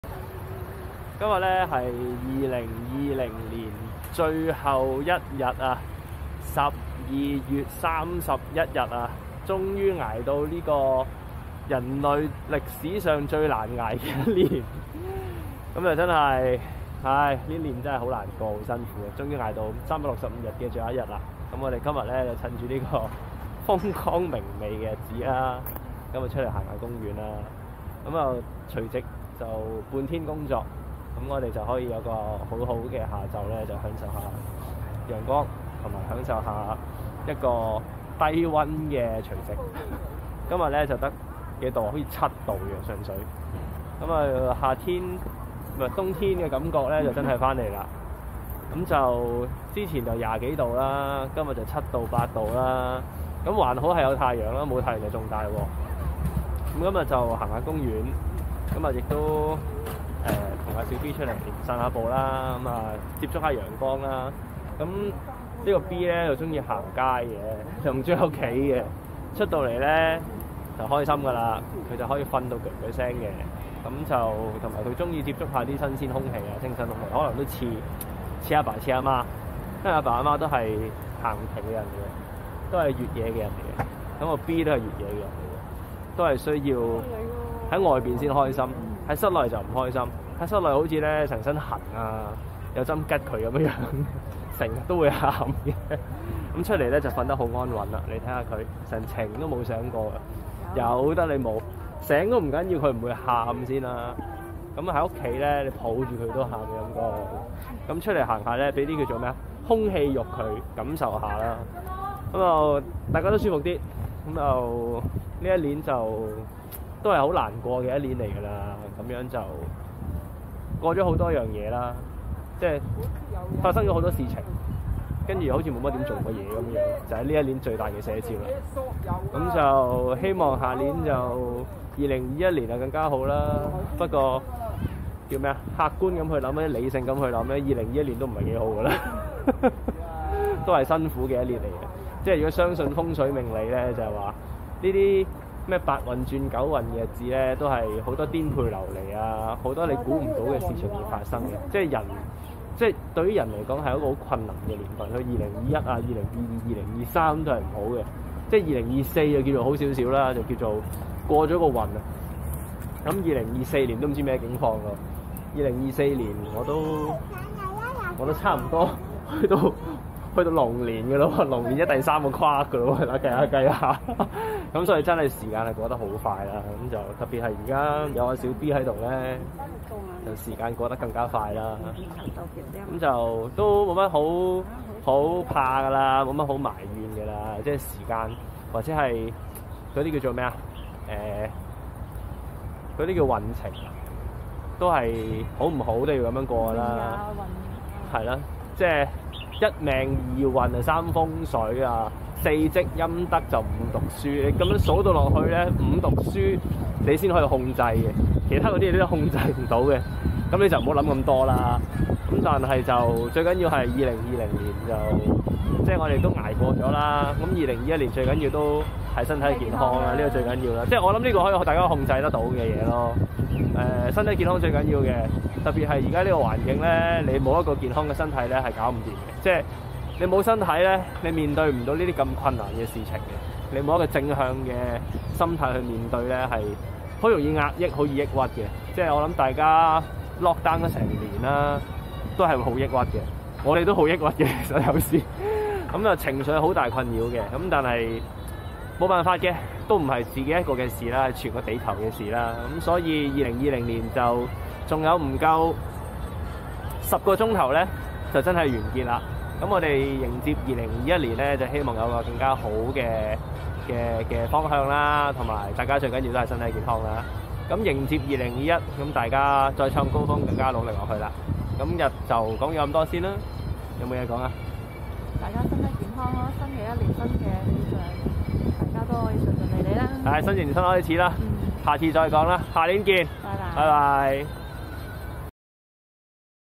【收皮啦2020】 終於挨到2020年最後一天，我們永遠唔好再見👋啦
2020認真經驚心動魄，一年咁就過左，好似無做過野，但事實上發生左好多野。理性上認為2021年都唔會容易過，但感性上都要希望在明天。

Powered by: Insta360 One R